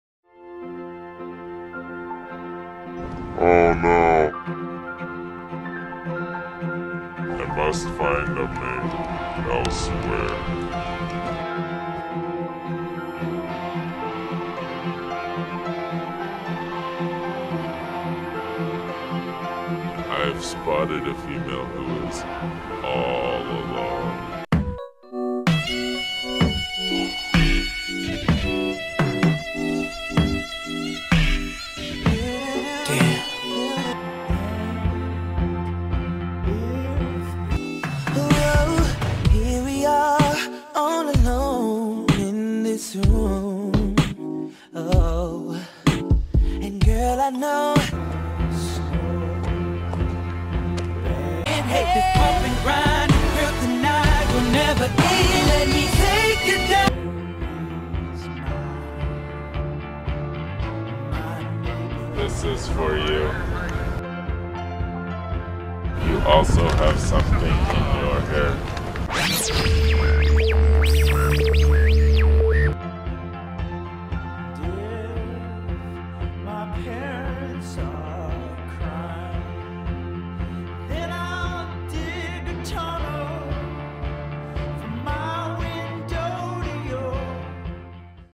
4. Michaela & Joyce (An Alien Love Affair: AiR Spring Show 2011)
Officer Michaela finally finds a female who is all alone. Joyce and Michaela then proceed to get married.

Artists in Resonance Co-ed A Cappella
Spring Show 2011: An Alien Love Affair
Wednesday, April 20 & Thursday, April 21